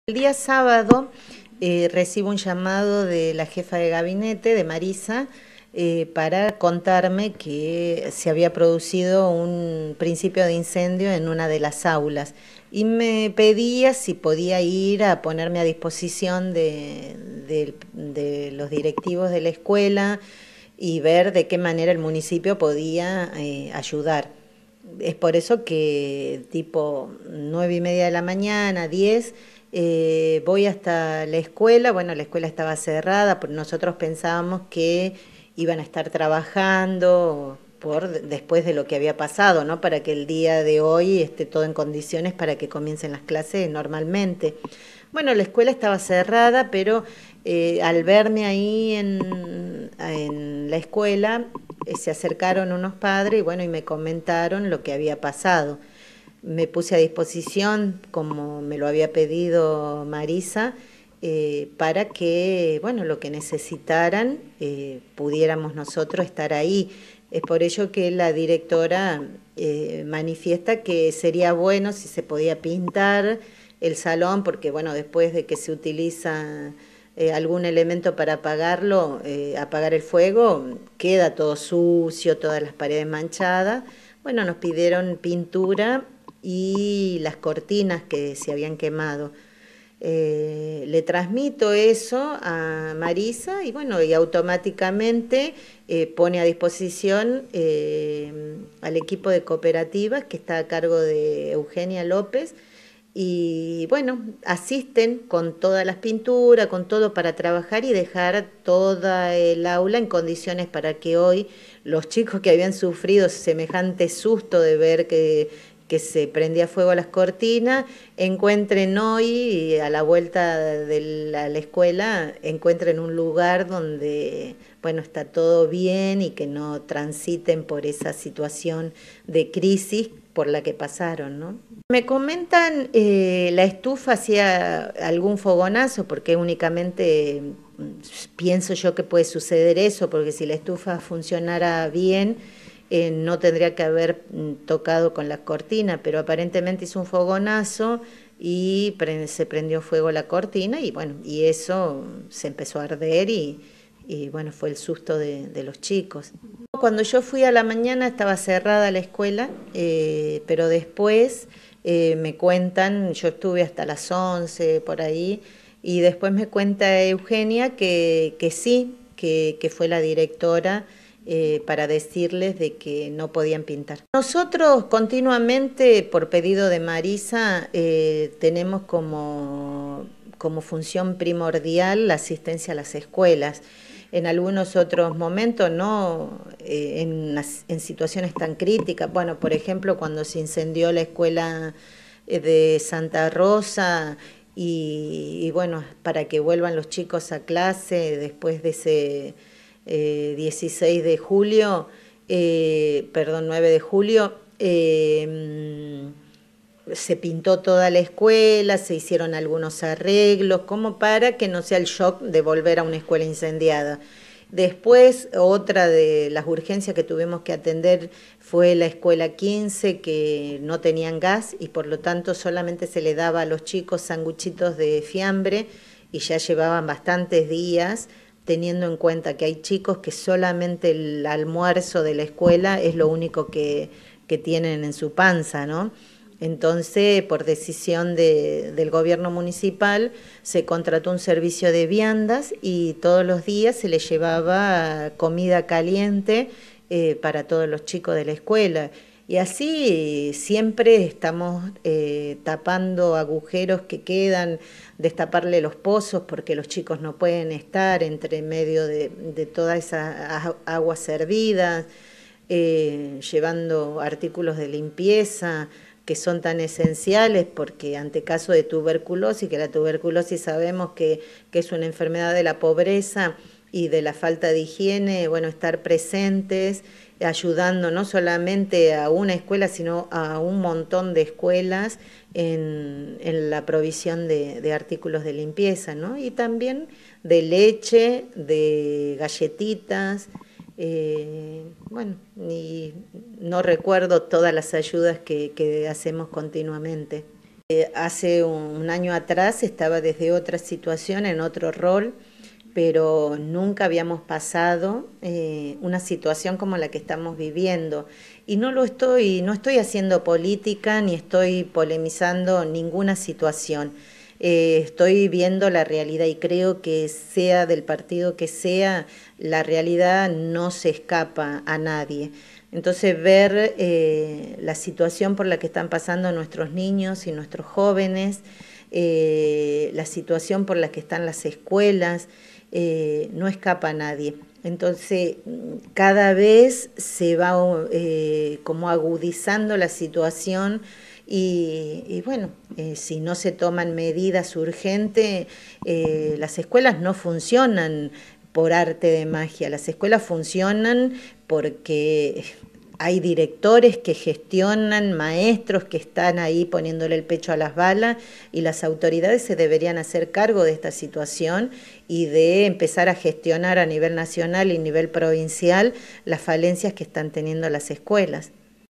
"Nuestra función primordial es asistir a las escuelas"
Sandra Cardozo, a cargo de la Dirección General de Ciencias, Tecnología y Políticas Educativas, habló con NacPop TV luego de lo ocurrido en la Escuela N°4 de Uribelarrea, el pasado fin de semana.